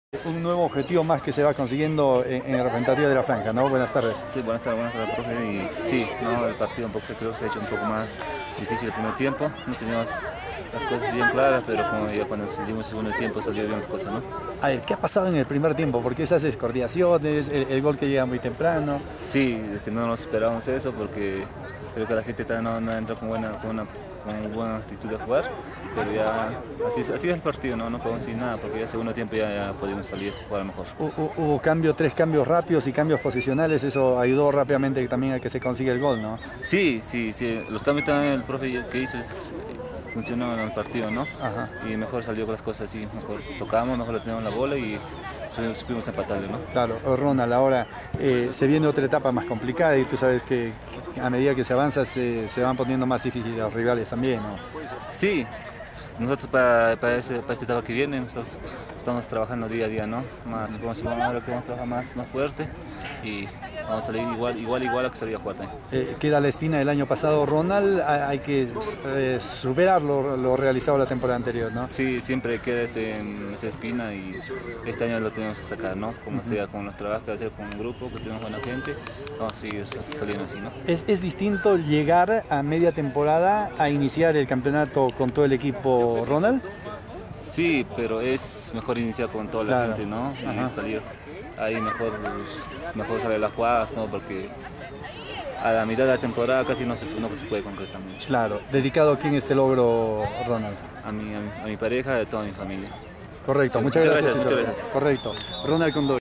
RONAL CONDORI QUICAÑO
RONAL CONDORI QUICAÑO, DEFENSA DEL ALFONSO UGARTE DE PUNO POR LA 6TA FECHA DE LA COPA PERÚ ETAPA PROVINCIAL PUNO 2019, JUGADO EN EL ESTADIO ENRIQUE TORRES BELON - ALFONSO UGARTE 2  POLICIAL SANTA ROSA 2 - GOLES DE DENNIS MAMANI Y LUIS PINEDA PARA POLICIAL Y ALBERTO VELA Y CARLOS ESCALANTE PARA EL ALFONSO UGARTE DE PUNO